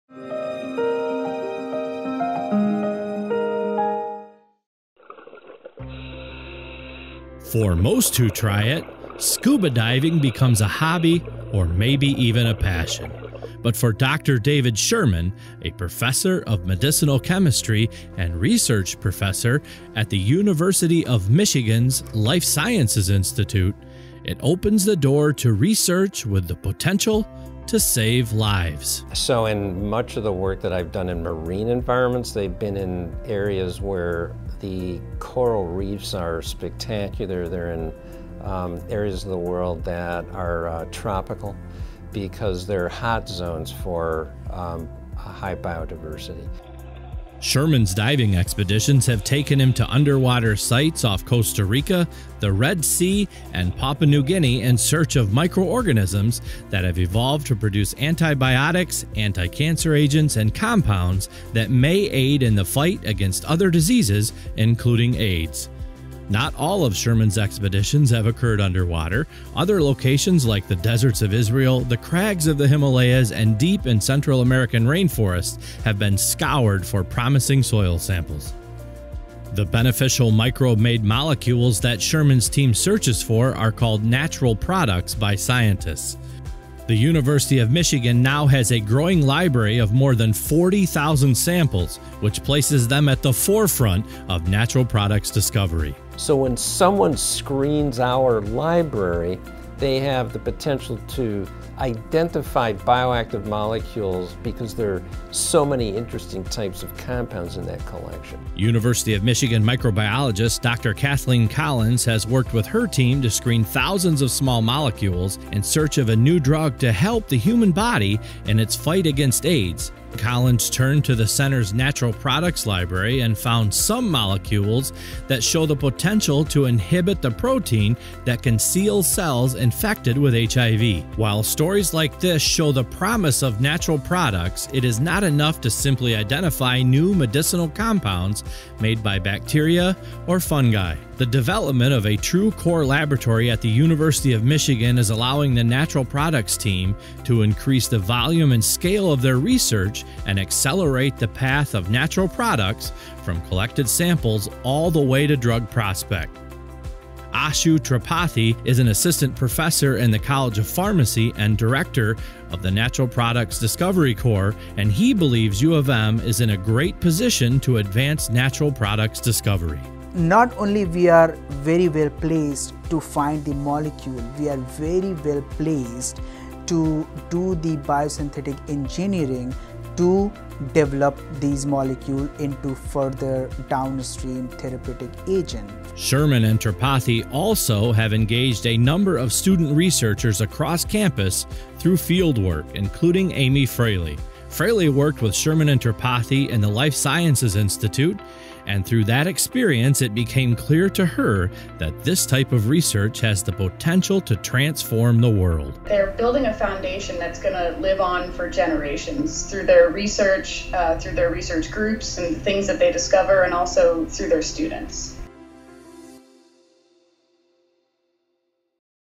Diving for Discovery
Some researchers sport lab coats. David Sherman prefers a wetsuit. The University of Michigan medicinal chemist and avid scuba diver travels the world in search of marine microorganisms to help pioneer new antibiotics, anticancer drugs and other medicines.

Created by Eric Shaw for @UMichResearch within the Office of Research at the University of Michigan.